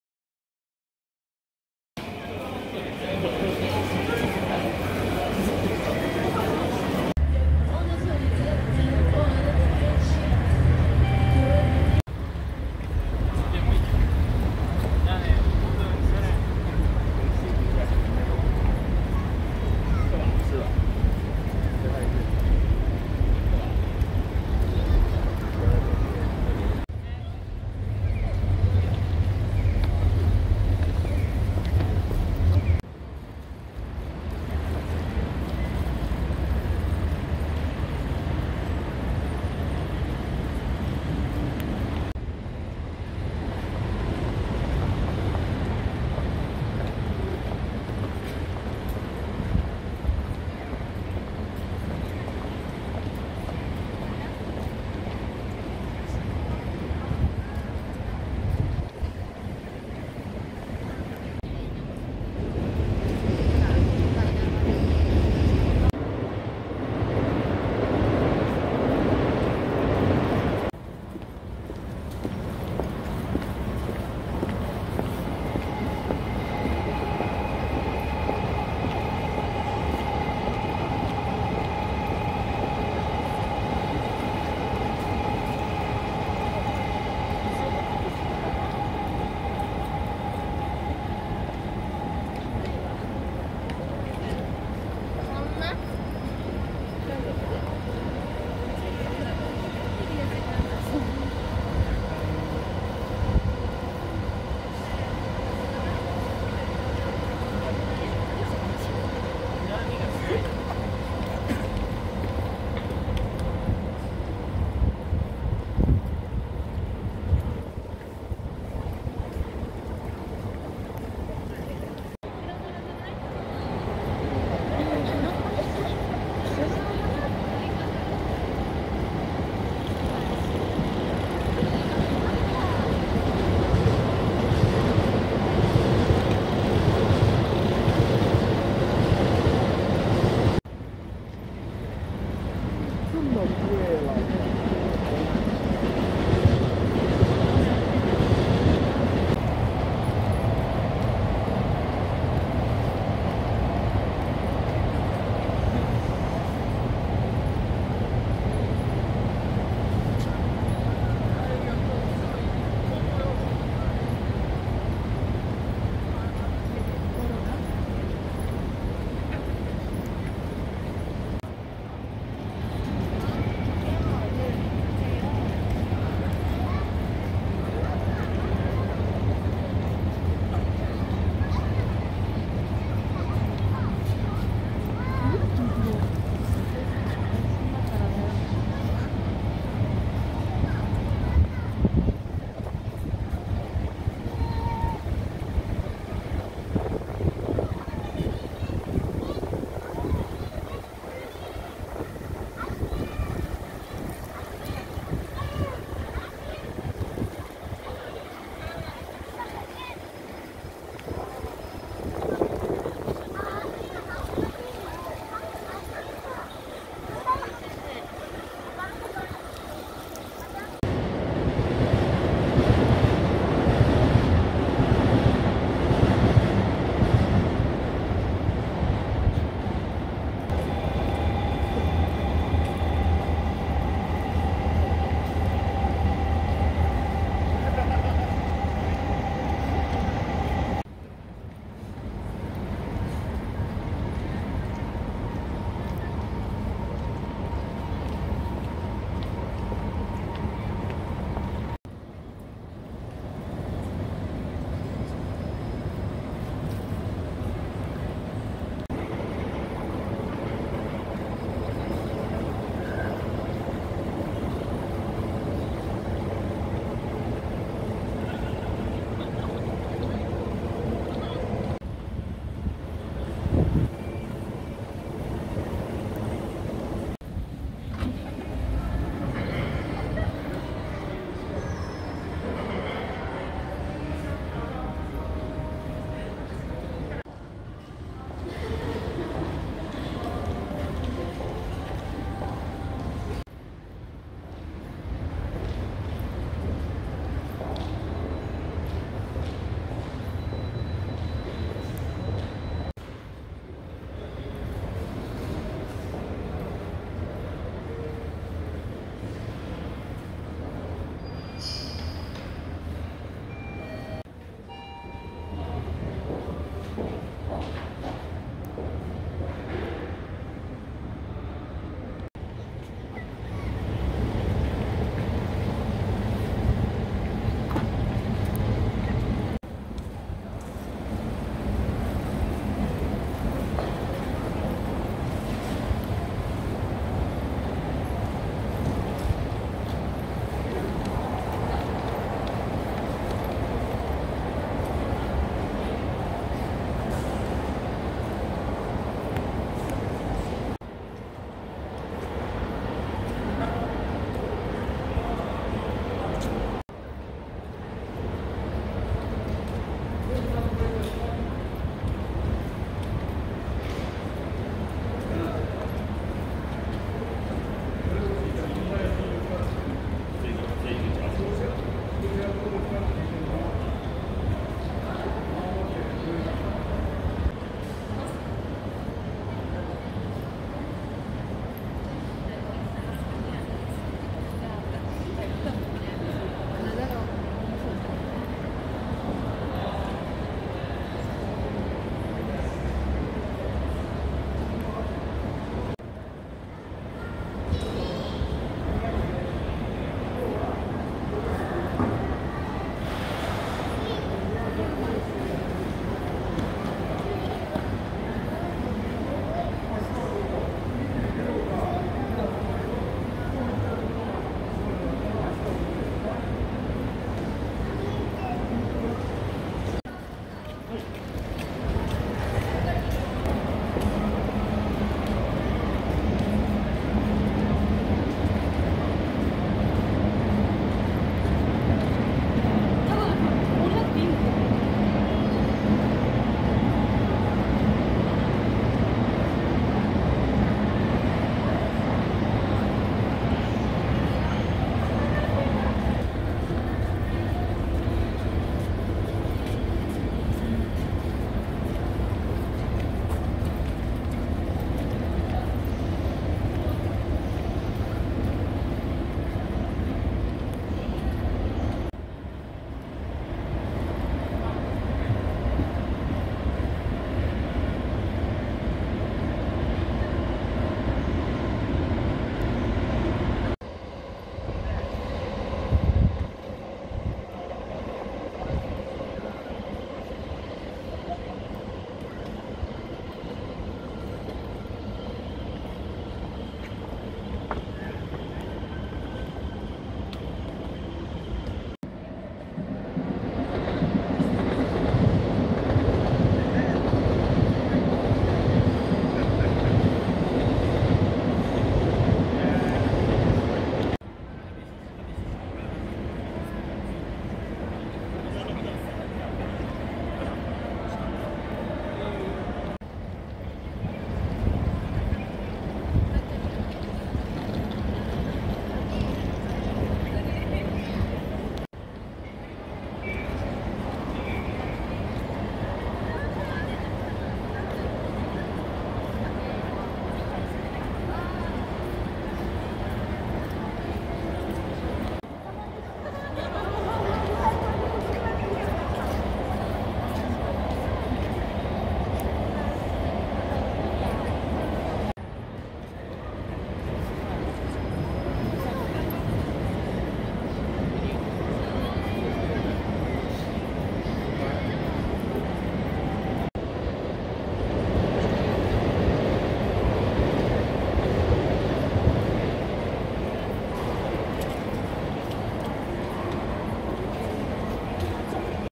Shinjuku Minamirumi（新宿ミナミルミ）2022
2022年11月30日、新宿南口の「新宿ミナミルミ」が3年ぶりに開催されているということで行ってきました。サザンテラスのイルミネーションのテーマは、宝石（Jewels in my Heart)ということです。
On November 30, 2022, I went to "Shinjuku Minamirumi" at the south exit of Shinjuku because it was being held for the first time in three years. The theme of the Southern Terrace illuminations is Jewels in my Heart.